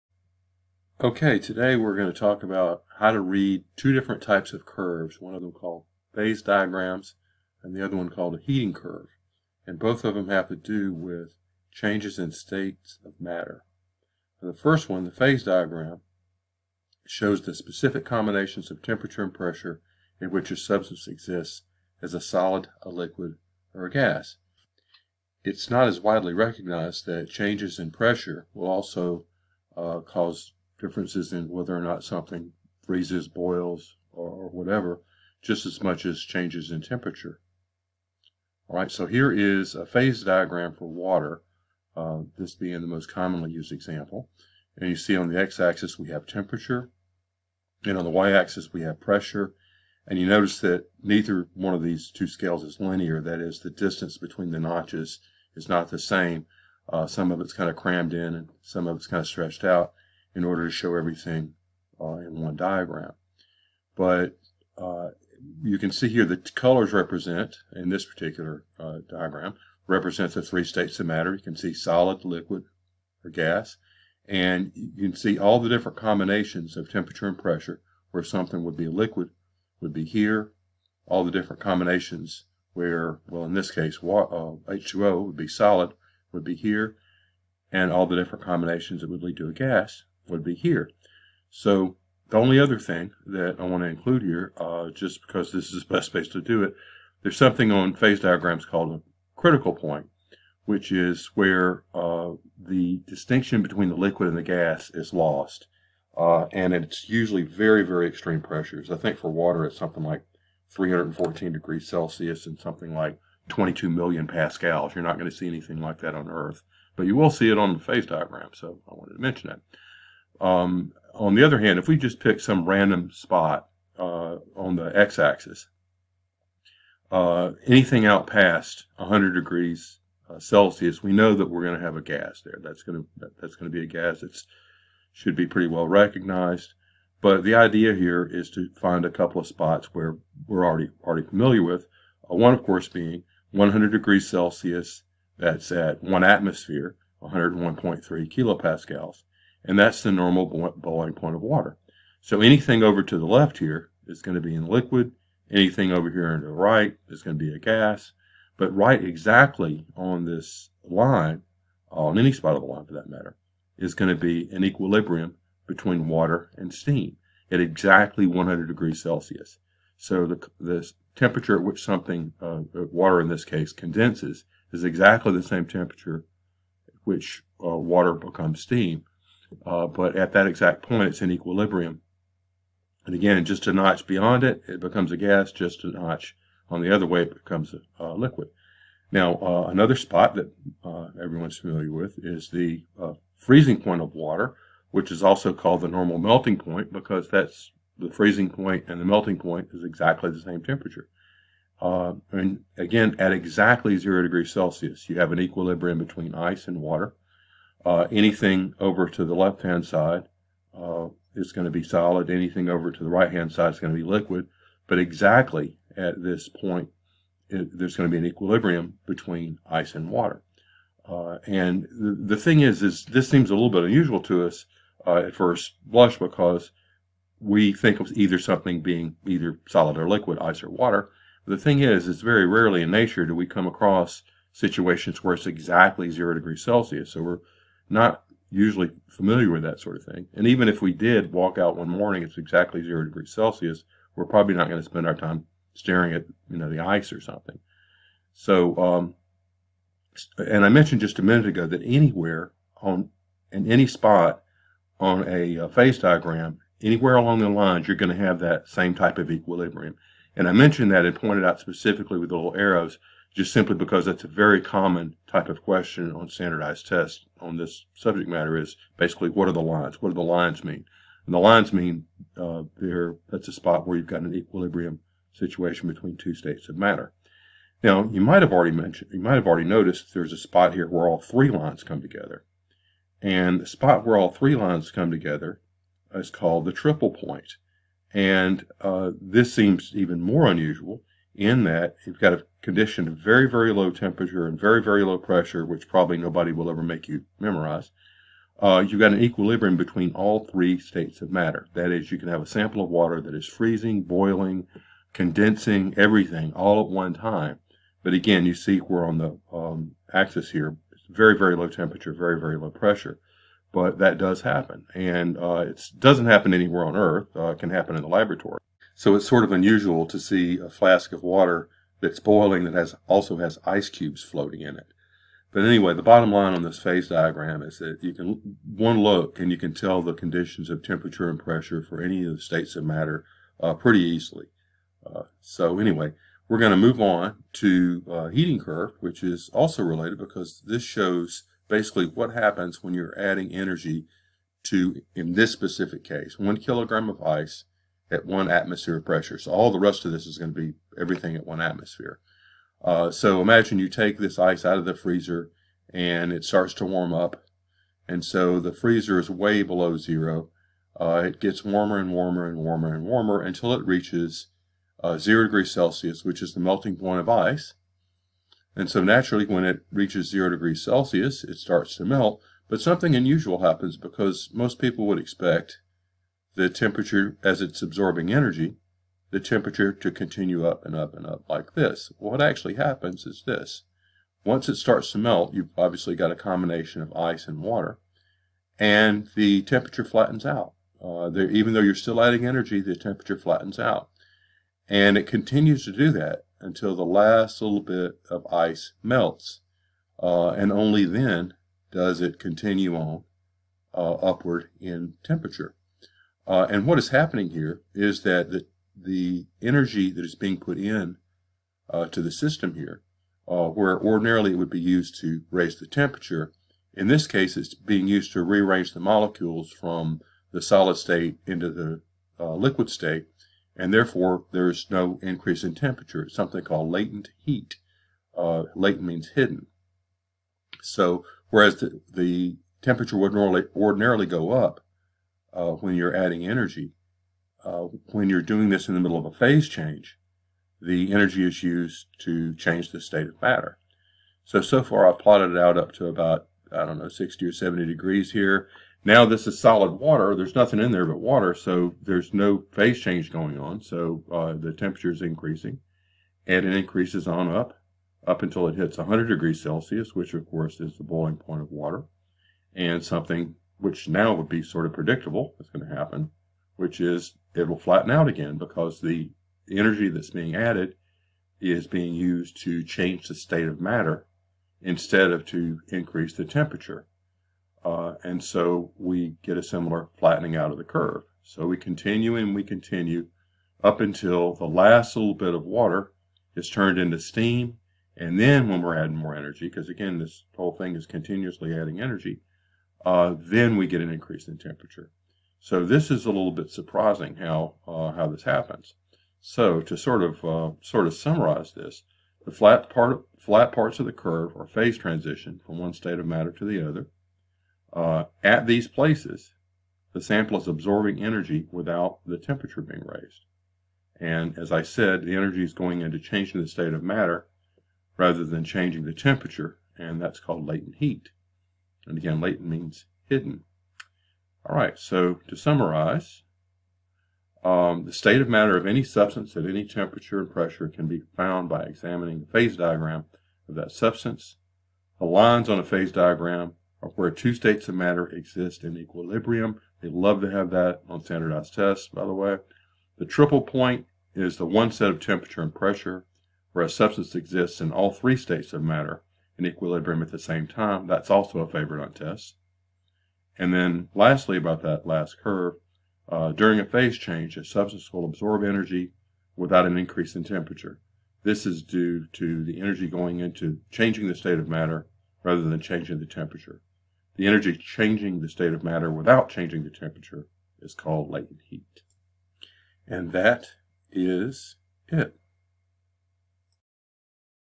How to Read Phase Diagrams and Heating Curves
The basics of the phase diagram and the heating curve are explained, including in the latter case, the concept of latent heat.  These are two different types of curves, but both have to do with what happens during phase changes.

0:00   Intro
0:14  Phase diagram overview
1:31 The critical point
2:20 Normal boiling point, normal melting point, and equilibrium
4:14  What are the lines on a phase diagram?
4:37  The triple point
5:36  Heating curve
5:55 Changes in temperature with changes of state
8:20 Heating curve and latent heat high points
8:44 Summary

Note that at just past 2 minutes or so I say that out past 100C water will be a gas, which is true in most cases, but the curve does show that at very high pressures water will still be a liquid at this temperature

-Jody Bruton